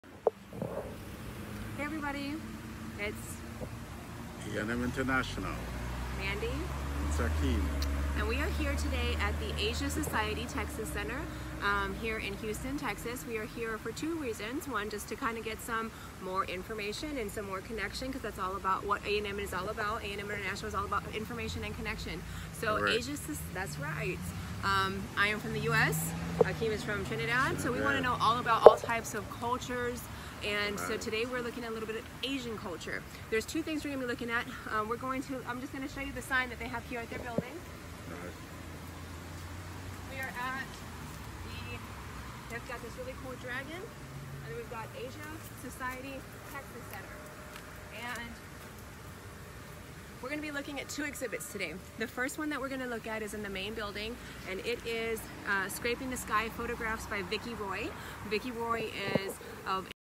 Art reception at the Asia Society Museum in Houston
Another mini adventure - we attended the reception for the exhibit: Tsuruya Kokei: Modern Kabuki Prints Revised and Revisited and got to see the photographs by Vicky Roy: Scraping the Sky.